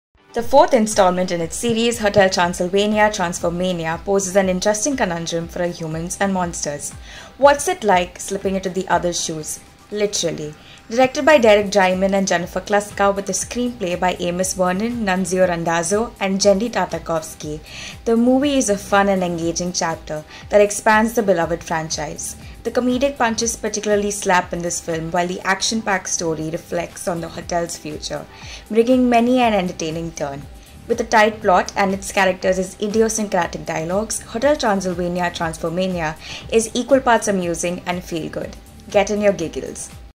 RSFlashReview: 'Hotel Transylvania: Transformania' (2022)
RS Flash Review is an all-new series where our contributing journalist Jessica Xalxo (@iridescentkaleidoscope) discusses and critiques movies, sitcoms, OTT shows and more, and here's the catch - all under 60 seconds.

What's the next flick you'd like us to review?